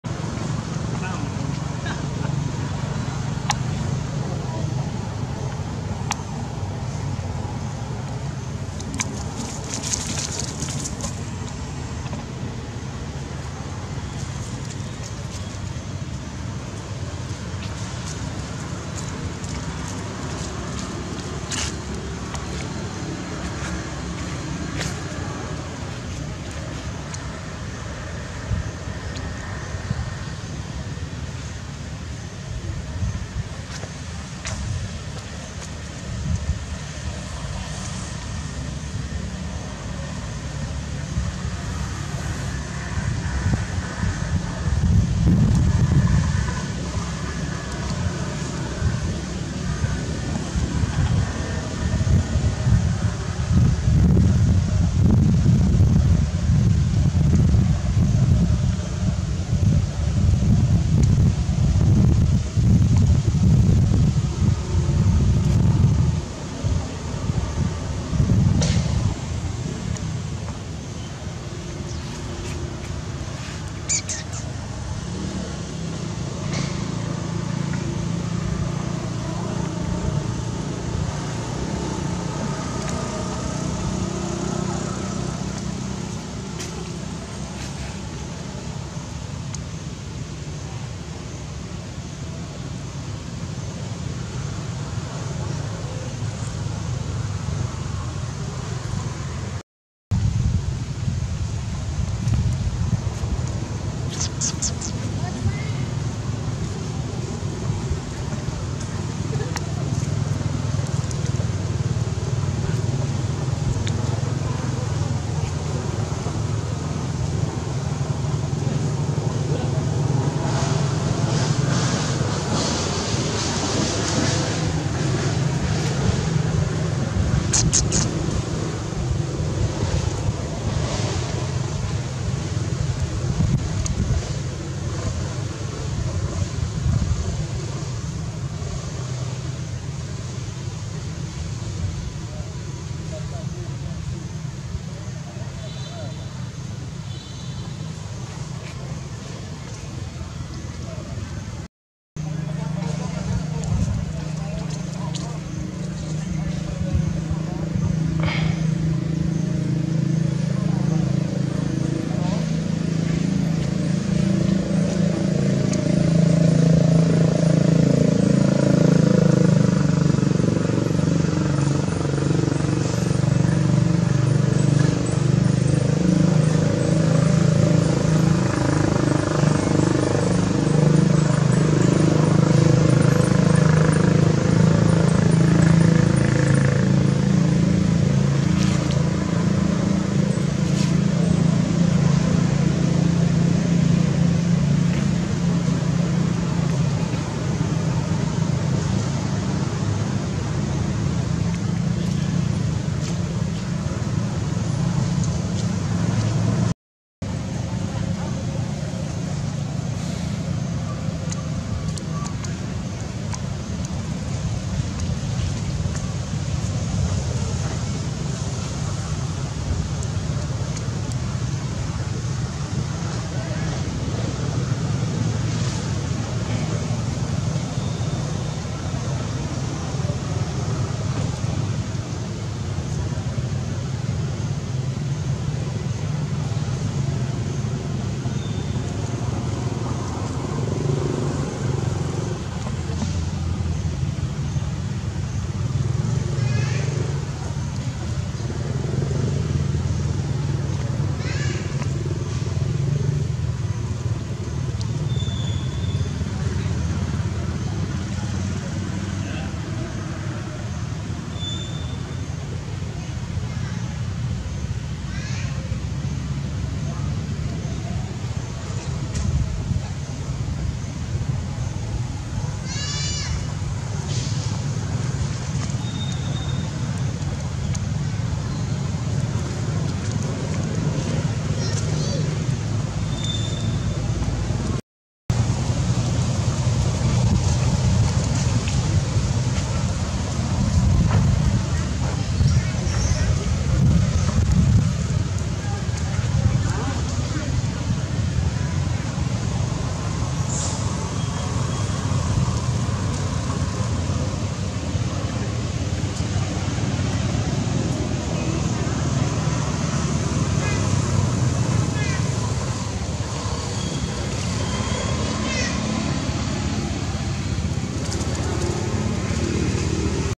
CALM DOWN PLEASE DELENA! POOR MACI WAS BITEN AND STEP BY YOUNGER AND SMALLER DELENA.
Today video is about:CALM DOWN PLEASE DELENA! POOR MACI WAS BITEN AND STEP BY YOUNGER AND SMALLER DELENA.